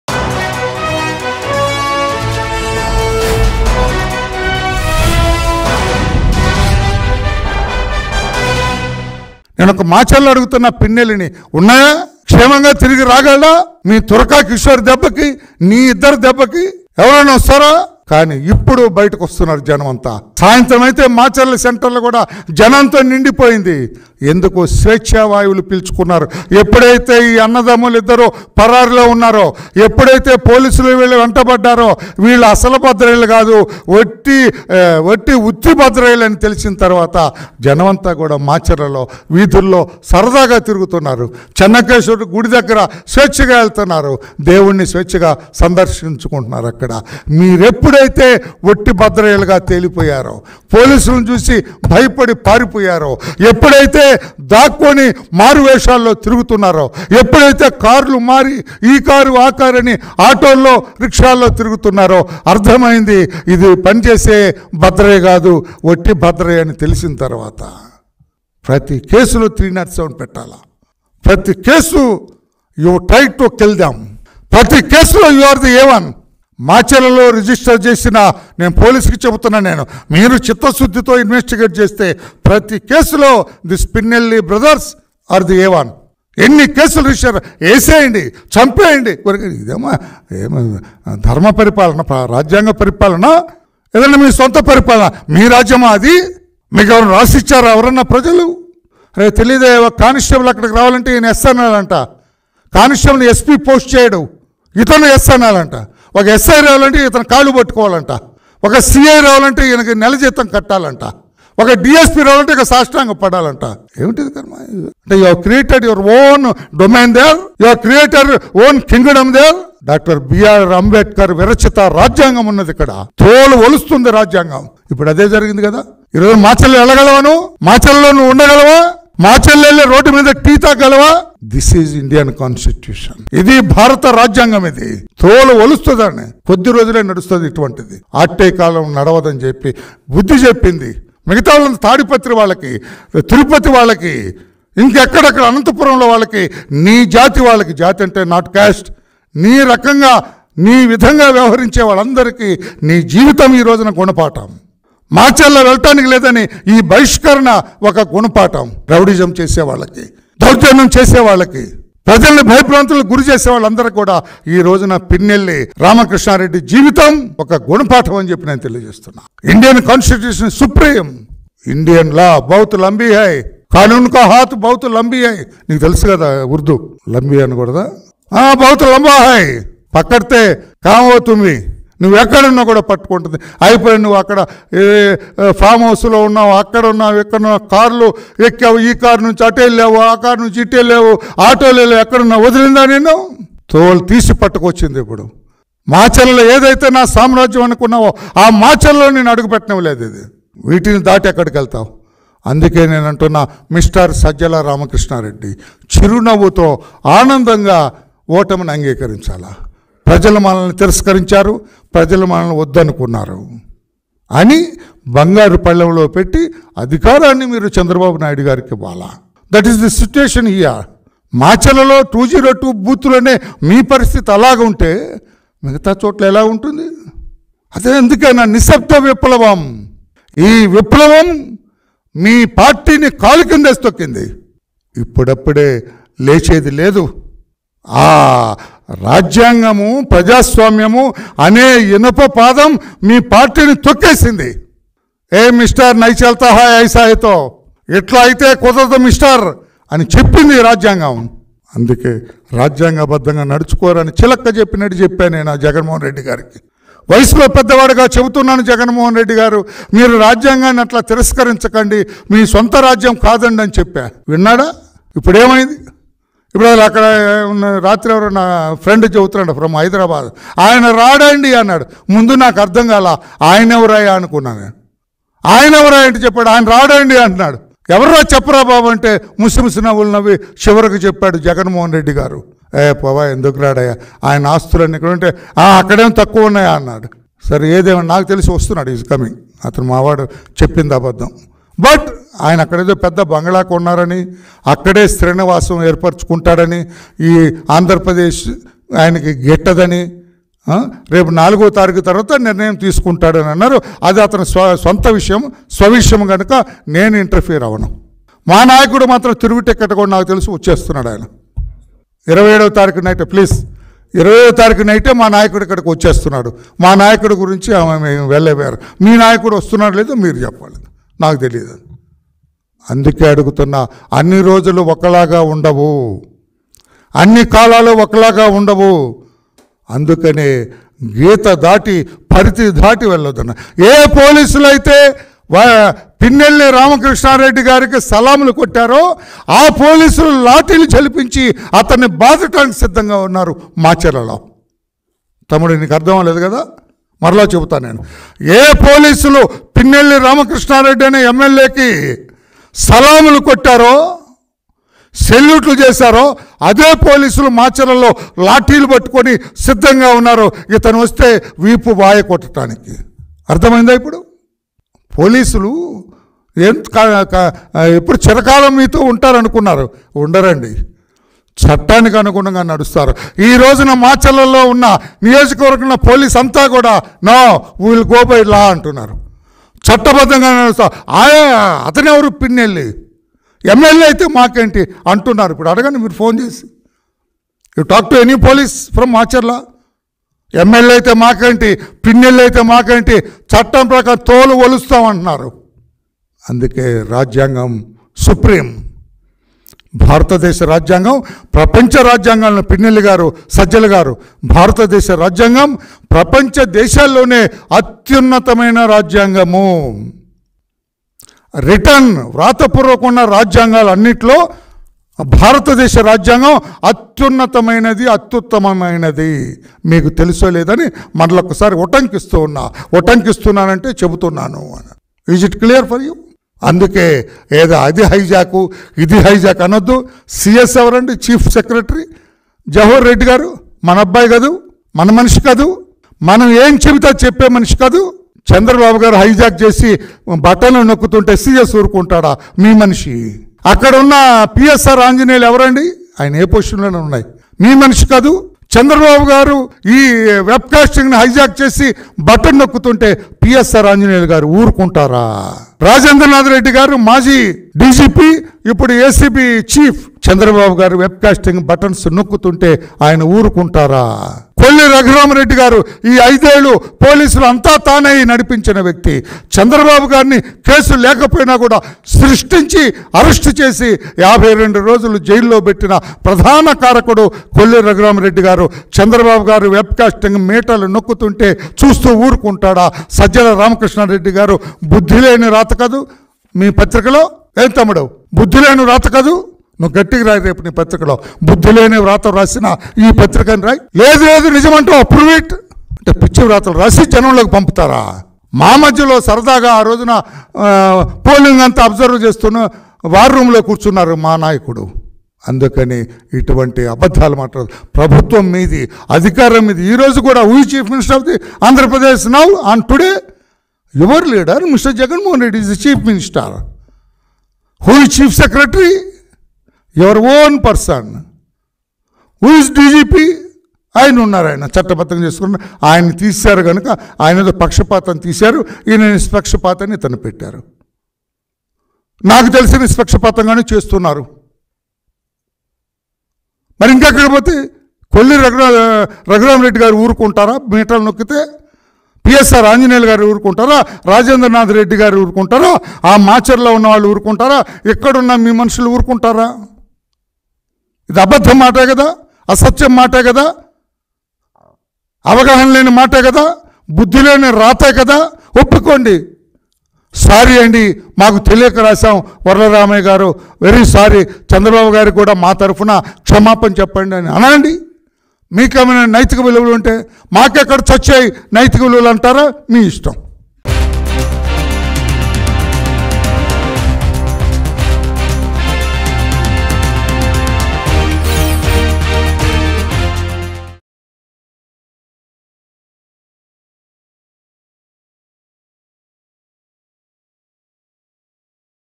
Varla Ramaiah Satirical Comments On Pinnelli Ramakrishna Reddy | TDP Vs YCP | AP Political News
Varla Ramaiah Satirical Comments On Pinnelli Ramakrishna Reddy. He slams Pinnelli Ramakrishna Reddy for seeking a stay on the case of EVM machine destruction.